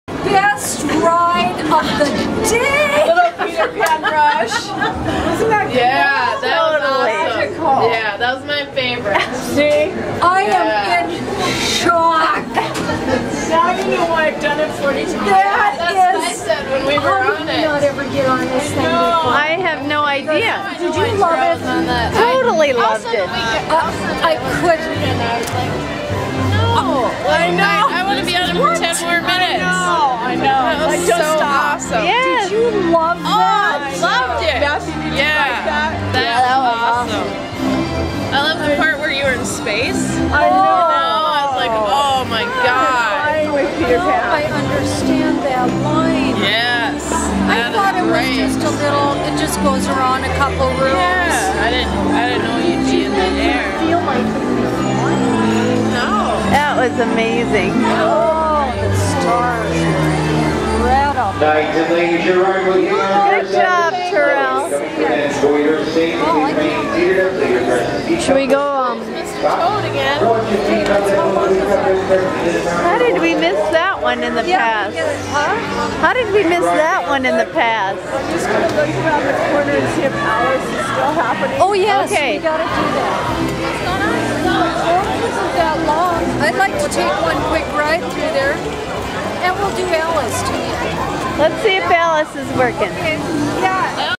Mom and the Peter Pan Ride - Disneyland
Mom coming out of the Peter Pan ride at Disneyland at about 11:45 pm!